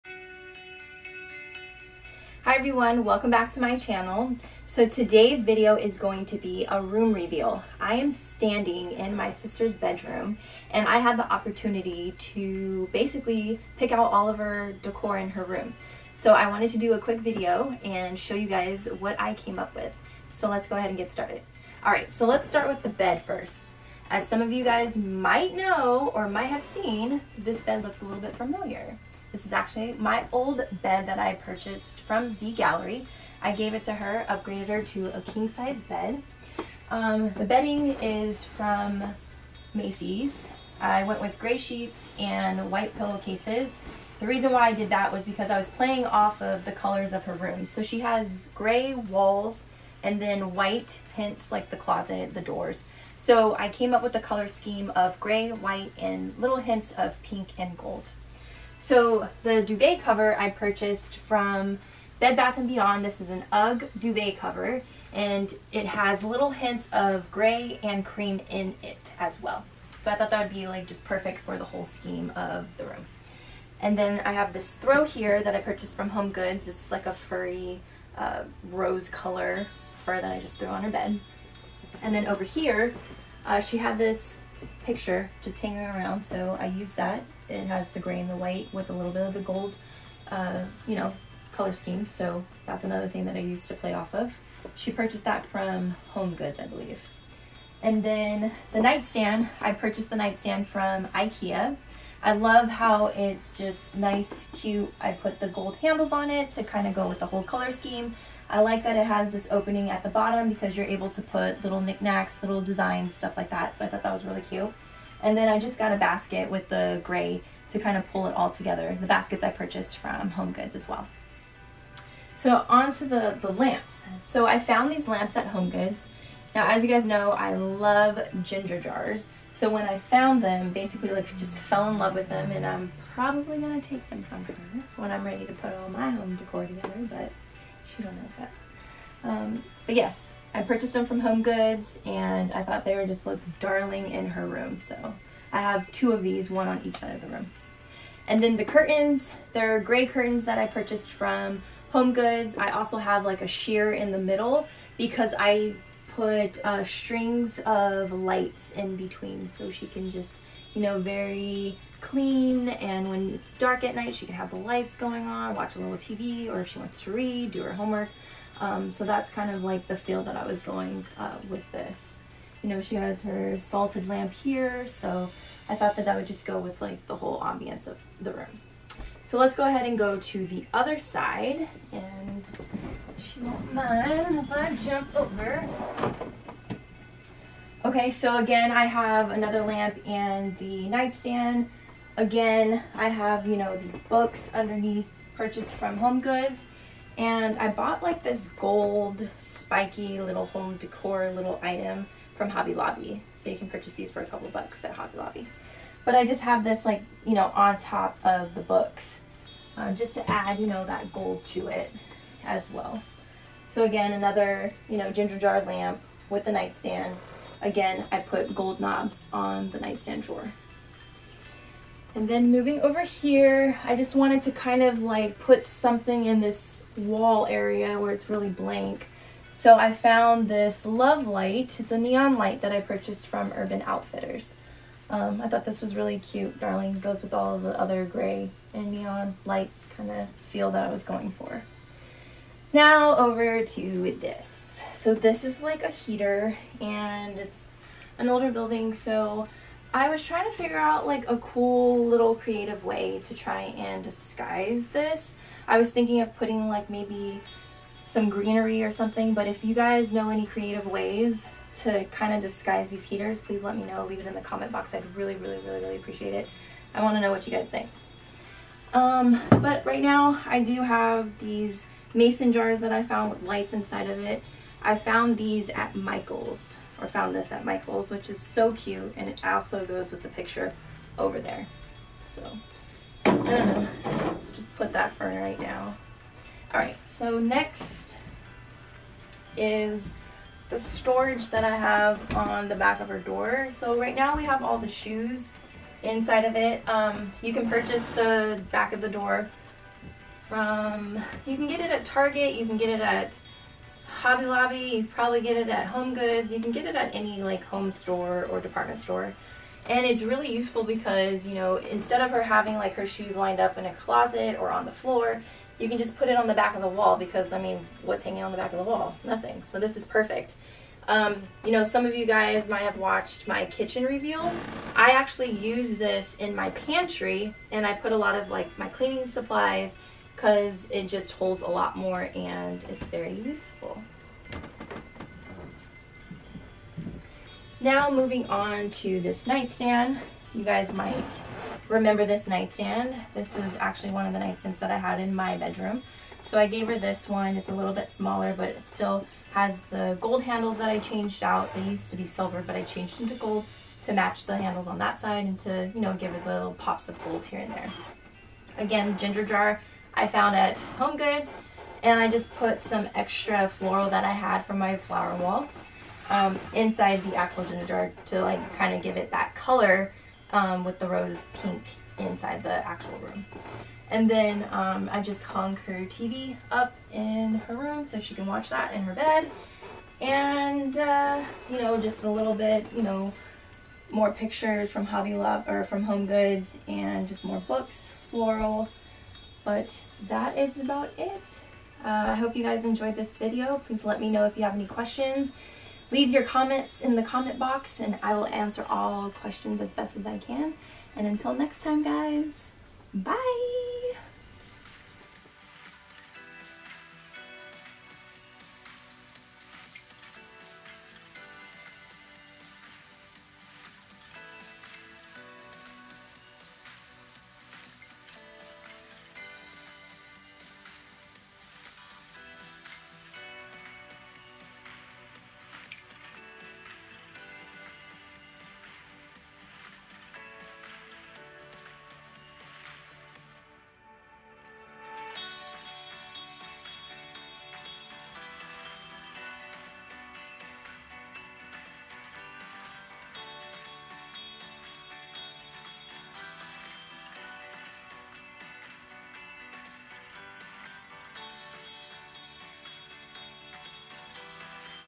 DECORATING MY SISTER'S BEDROOM - BEDROOM REVEAL I STACIA KARCHER
So I had the pleasure of having my sister trust me with decorating her bedroom. This is a quick video of the colors and decor I used. i also give a breakdown of where I purchased everything from. Please let me know if you have any idea's on hiding those old ugly heaters in the rooms! Hope you enjoy!

Ciao!!!

XOXOX
Stacia